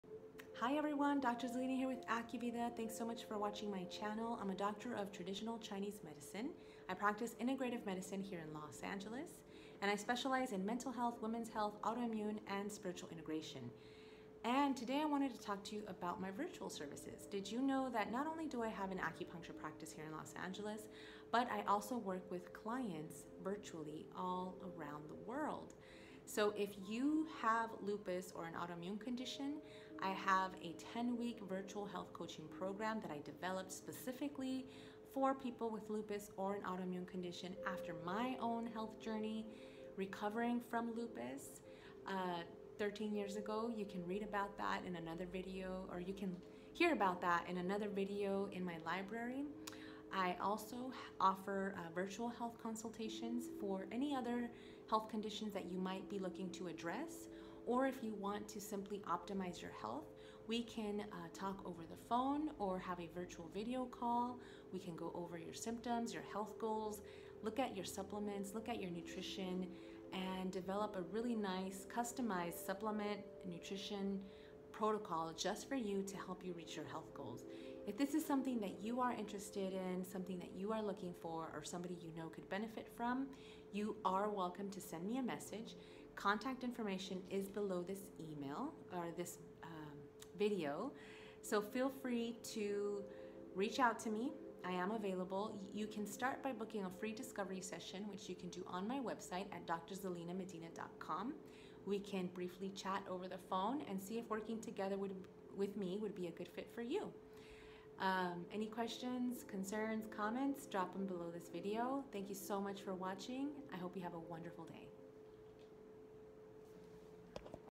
Virtual Services
Did you know that aside from providing in person treatments, I can also work with you virtually?
Wherever you might be…

I offer virtual services to help you reach your health goals.

I can help with:
Nutrition
Supplements
Herbs
Lifestyle Medicine and more…